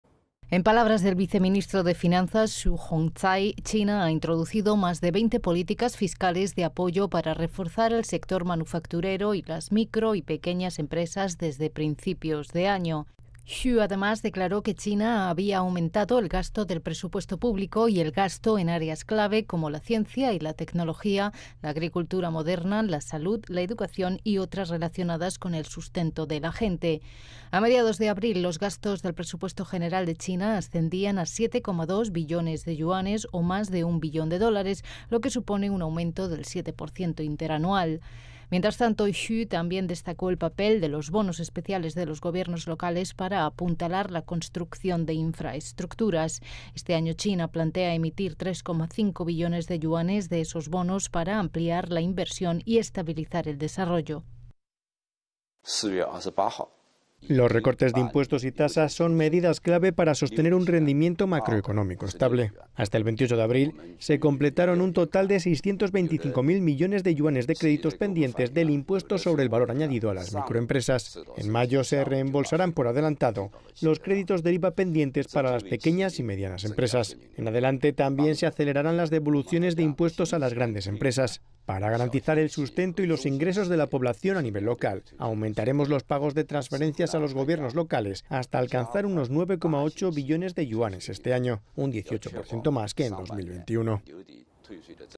China introduce medidas fiscales proactivas para estabilizar la economía
China ha introducido medidas fiscales proactivas y de varios tipos que persiguen estabilizar la economía. En palabras del viceministro de Finanzas, Xu Hongcai, China ha introducido más de 20 políticas fiscales de apoyo para reforzar el sector manufacturero y las micro y pequeñas empresas desde principios de año.